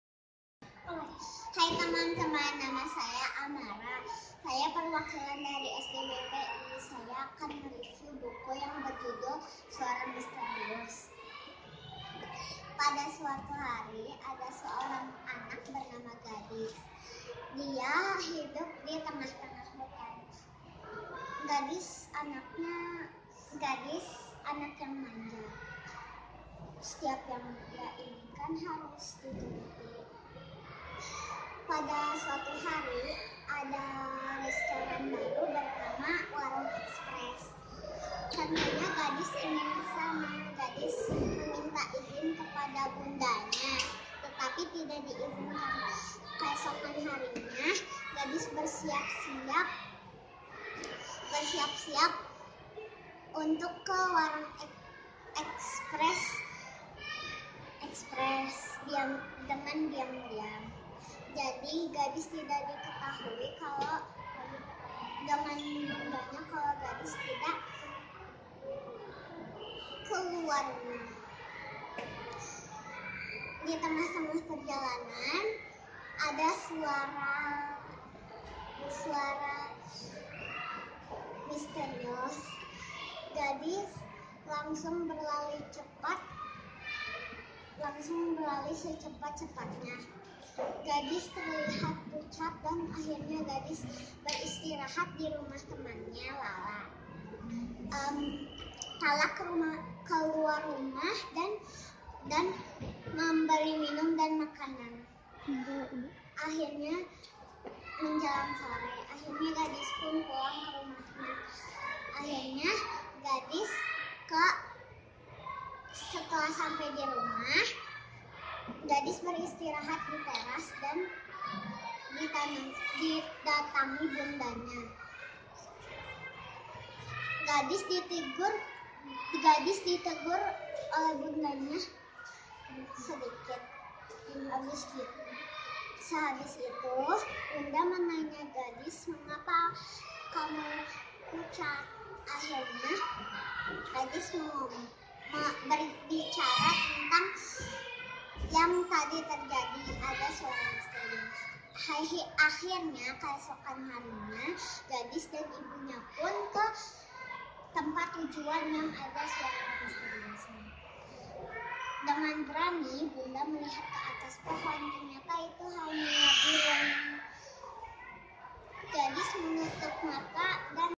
Review Buku "Suara Misterius" SD BPI Kota Bandung
Review Buku "Suara Misterius" oleh salah satu peserta WJLRC yaitu Ammara Nathifa Ashavtya dari SD BPI Kota Bandung